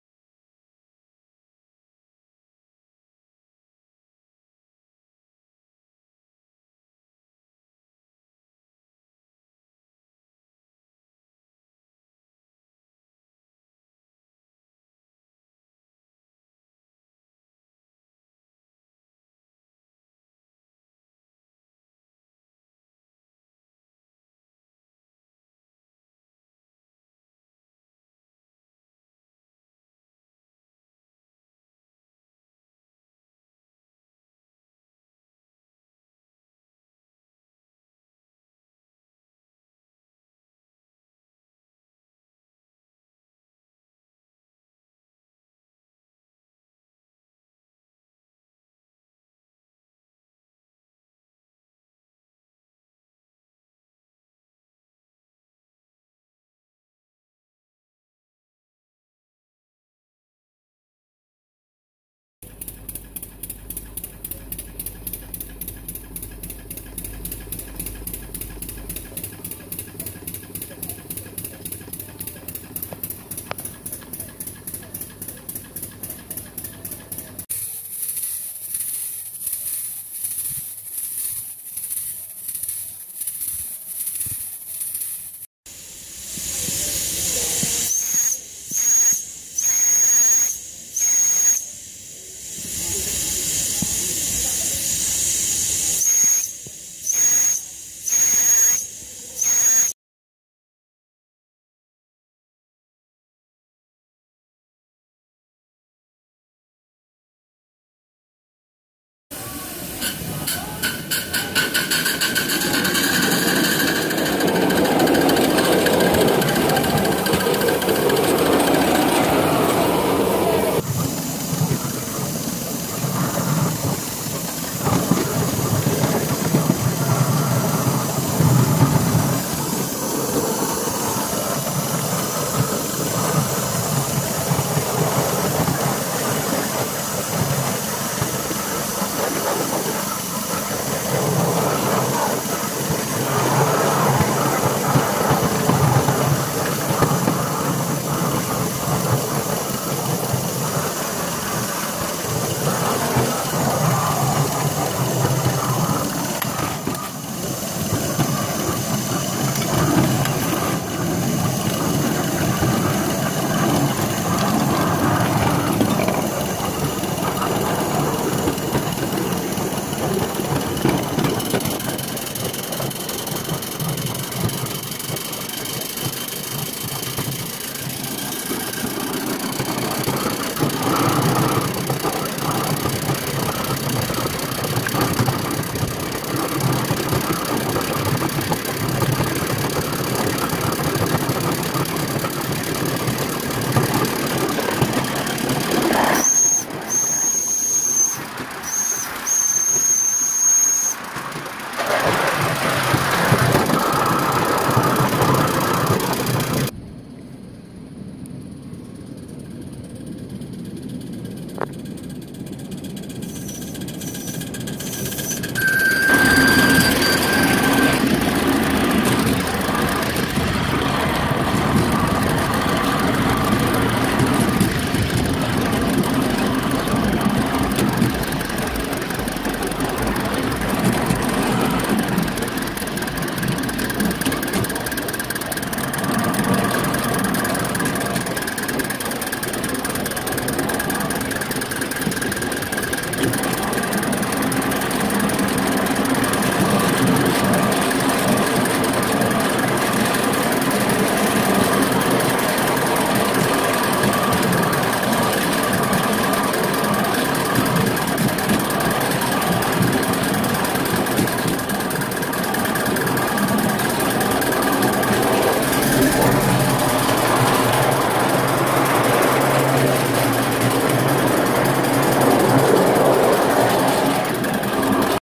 Ballaarat video
A live steam model of the Ballaarat, A Western Australia timber loco.
This loco was designed and built during lock-down, taking 9 months from first casting to first steaming.
Other than a lathe and some HSS cutting tools, this loco can be built in a very modest workshop. Even the name plates are etched using a toner printer, copper sulfate and an old cell phone charger.
The scale hauling loads and speeds in the video are roughly 1ton and 160kph respectively, not bad for a 150 year old design. The little loco can haul heaver loads much faster, but we have necessary speed restrictions on our little track!